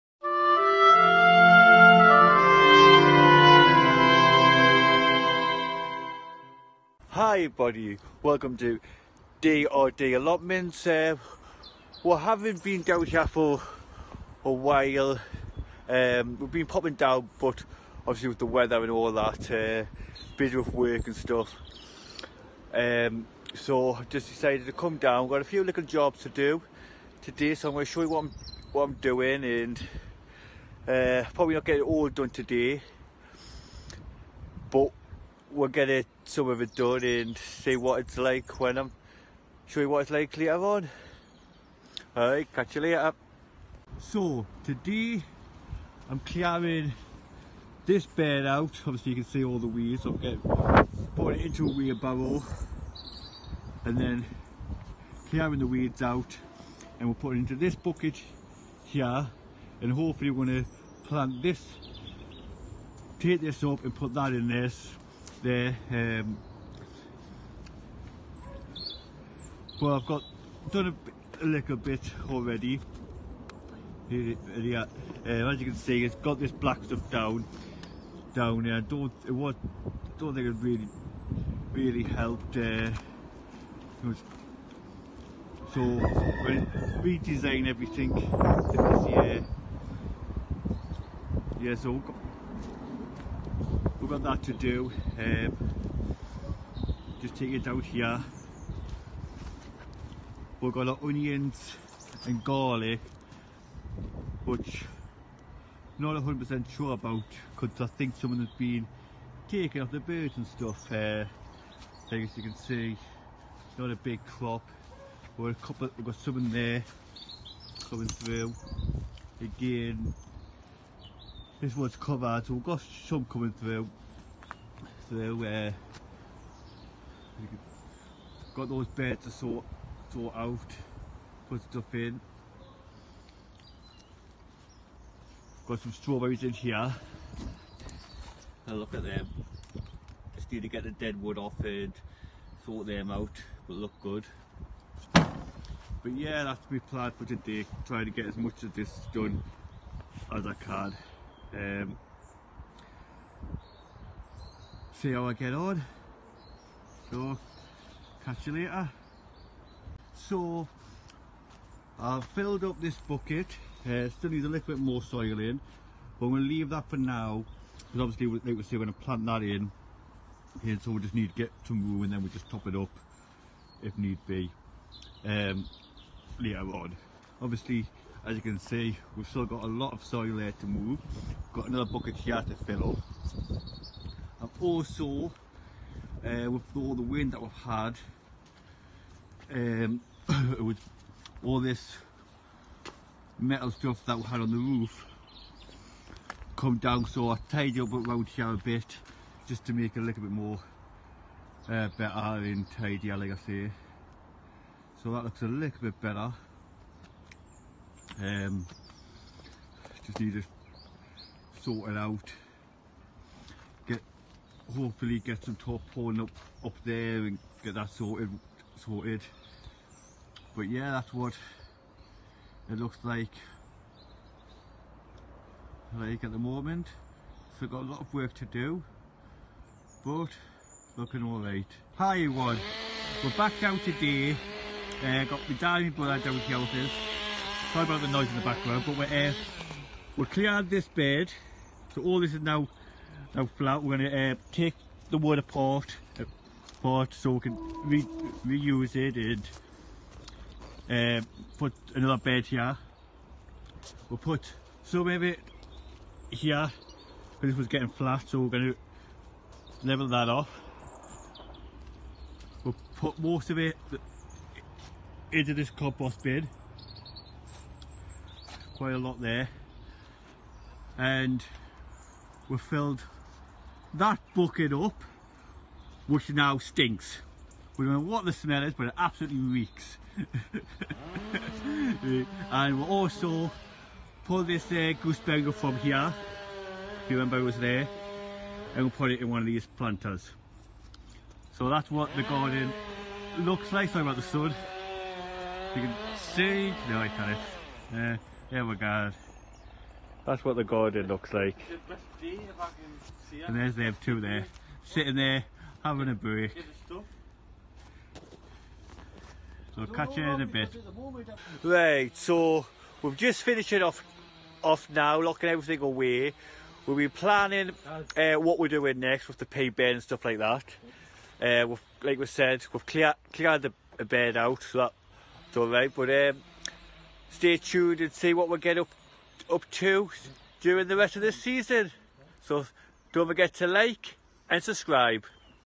Febuary 2023 at the Allotment
Tour of the plot as first frost hits us